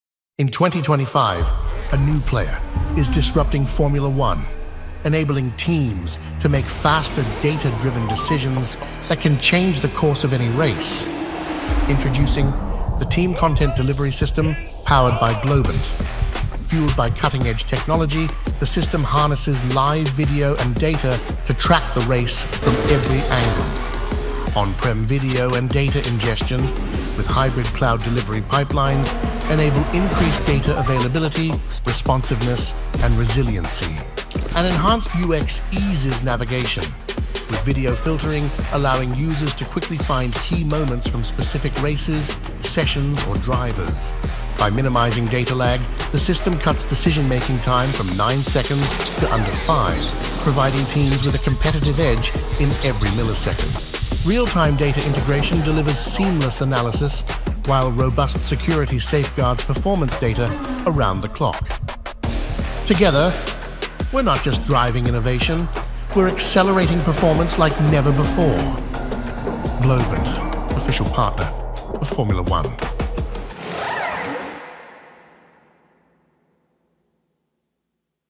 The System That’s Powering Formula 1® Performance | The TCDS Explained
In Formula 1®, milliseconds define victory.
Discover the Team Content Delivery System (TCDS) — a groundbreaking platform transforming how teams access how teams access race-critical data, insights, and video footage in real-time.
The TCDS enables:
 ⚡ Data + video delivery in under 5 seconds.
 🎥 Seamless access to 30+ video feeds.
 🧠 Advanced UX/UI for smarter decisions.
 🔐 Robust, secure infrastructure for every Grand Prix.

How is this technology shaping the future of racing? Learn about the latest F1® innovations in our Tech Report